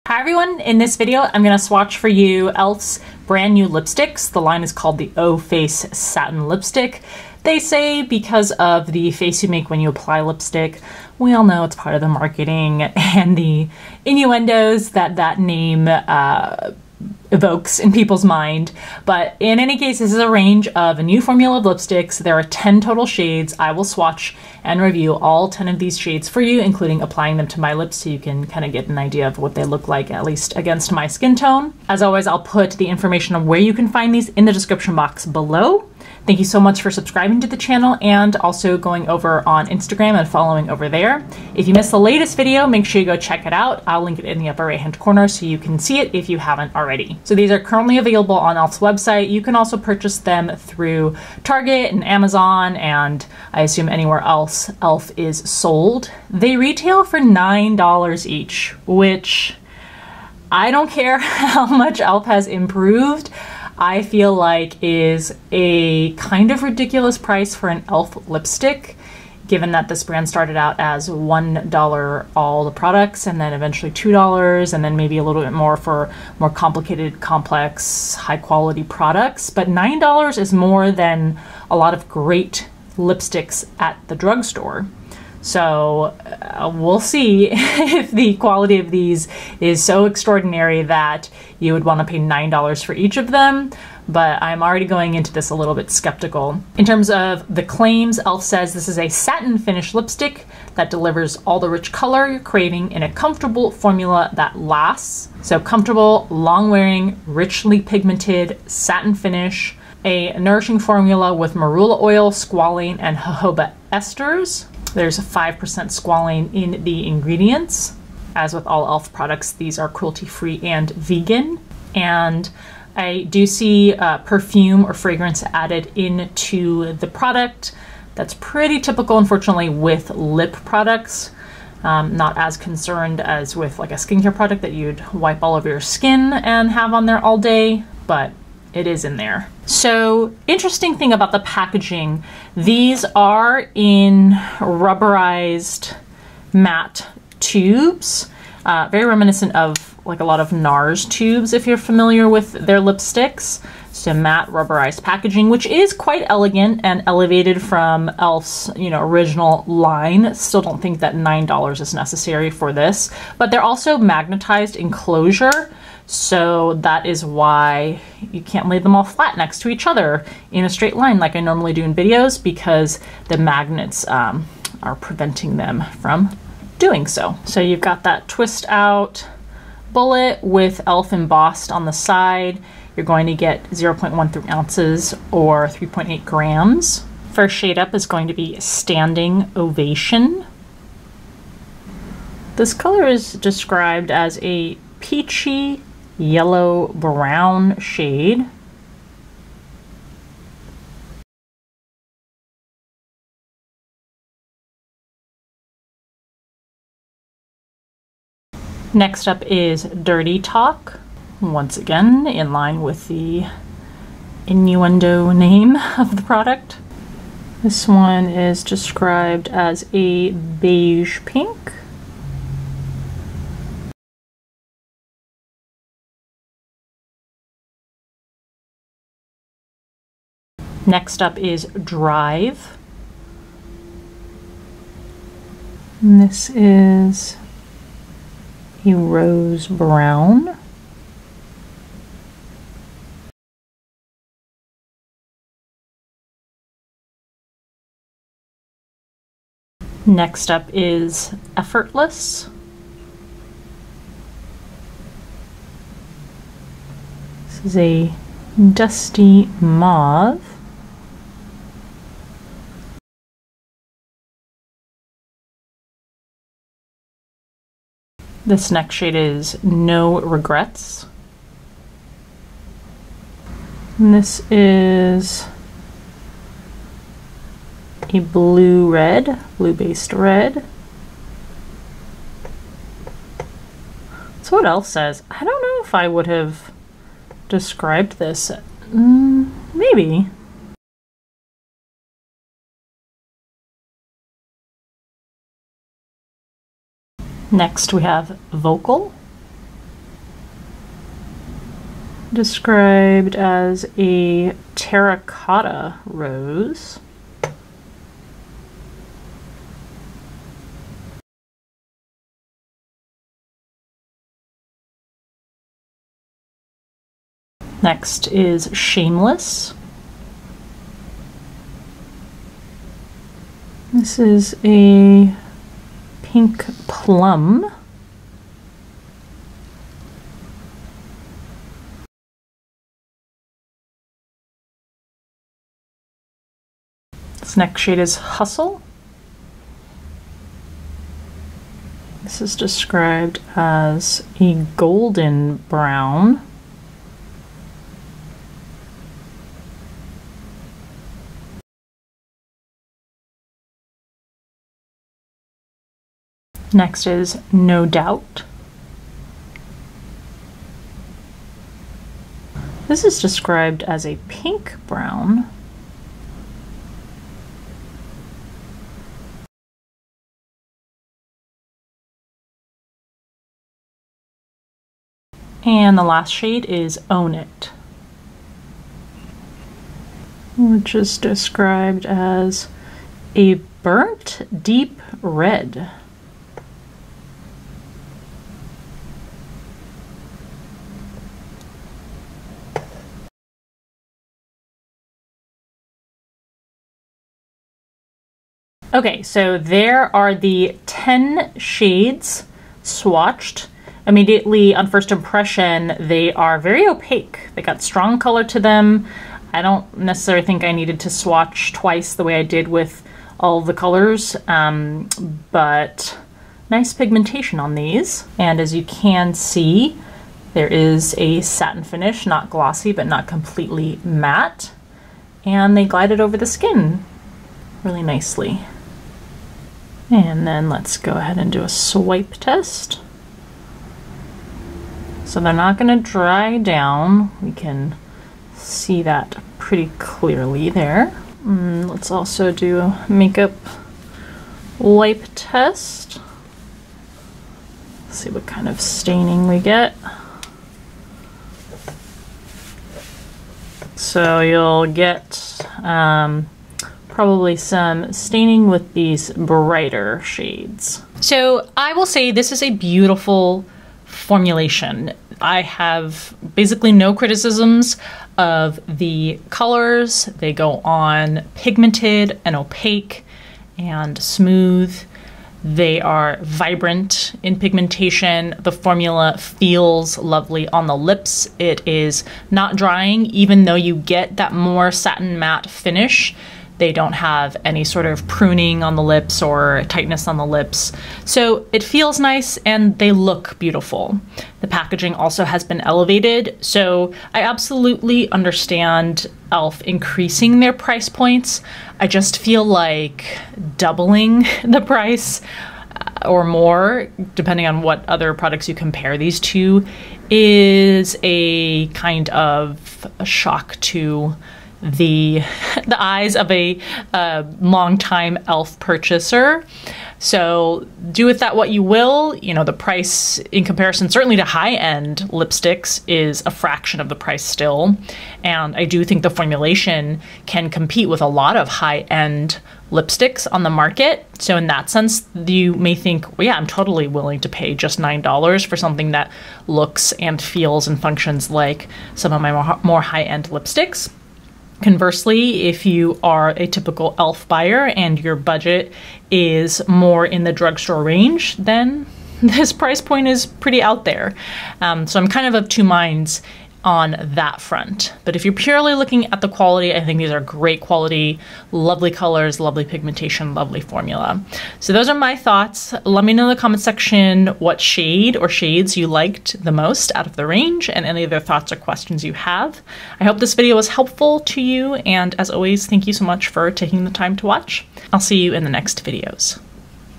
NEW elf O Face Satin Lipsticks // LIP SWATCHES & REVIEW
The best drugstore formula lipstick yet? All 10 shades swatched! 💋
➡️ Amazon Storefront *